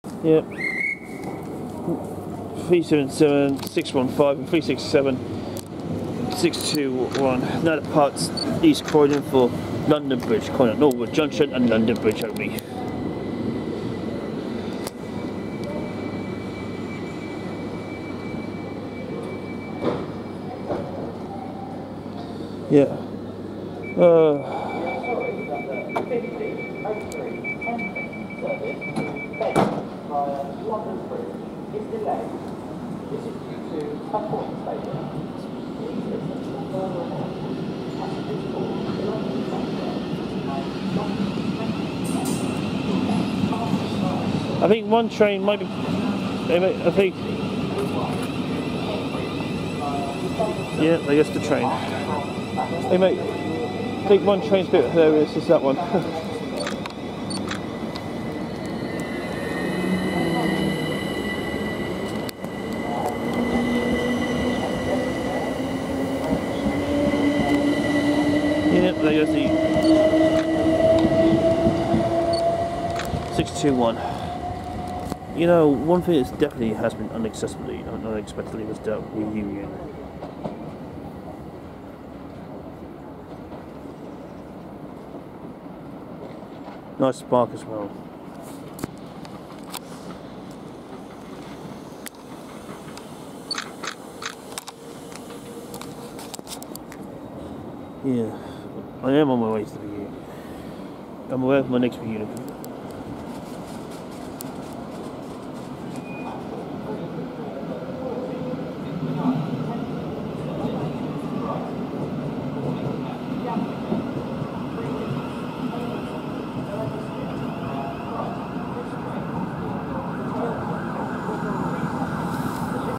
Southern Class 377 615-377 621 Departure East Croydon for London Bridge
Southern Class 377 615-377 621 to London Bridge Calling at Norwood Junction and London Bridge. And this train was from Caterham & Tattenham Corner to London Bridge on Tuesday 3rd December 2024.